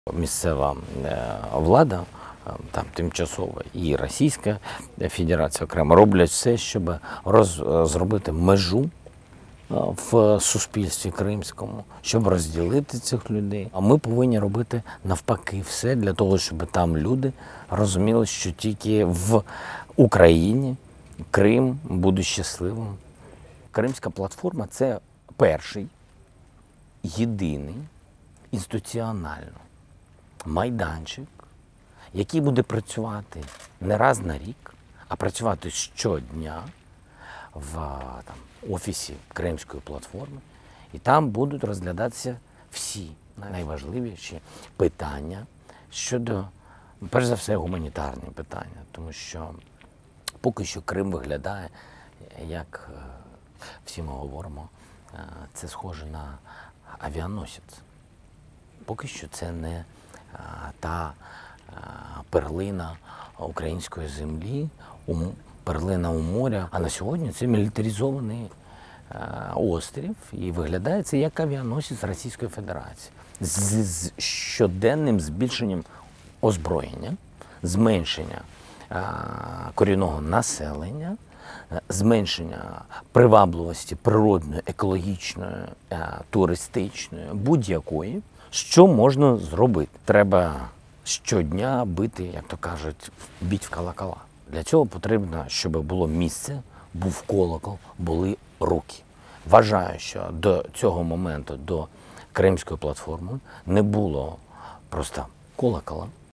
Zelenskiy Takes Aim At Russia Ahead Of International Meeting About Crimea
Ukrainian President Volodymyr Zelenskiy has accused Russian authorities of trying "to create a rift" within Crimean society as he tried to put the international spotlight back onto the Russian-occupied peninsula. Crimea has been occupied by Russia since 2014.